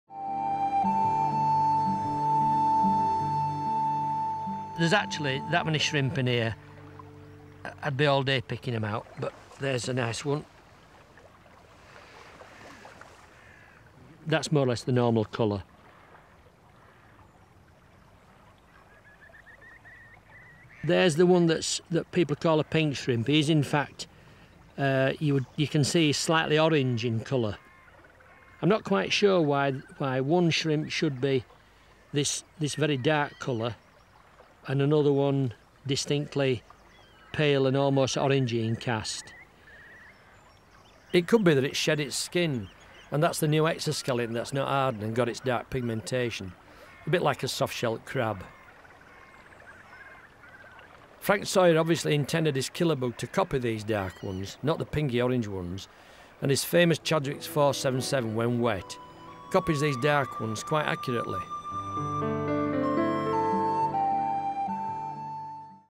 The Freshwater Shrimp, How to Sample your Stream
Oliver Edwards @LearnFlyFishing shows the vast number of shrimps he caught in just one sample. On alkaline rivers, like the chalk streams in the South of the UK, freshwater shrimps are found in vast numbers.  The Freshwater Shrimp comes in many sizes and shades, from a pinky-peach colour, through olive, to a drab grey.  Follow how Oliver samples in these entomology videos and then have a good copy at hand to match what you find in your stream or river.  How to tie the Oliver Edwards Deep Diving Shrimp with its tungsten weight is shown in another video on the channel.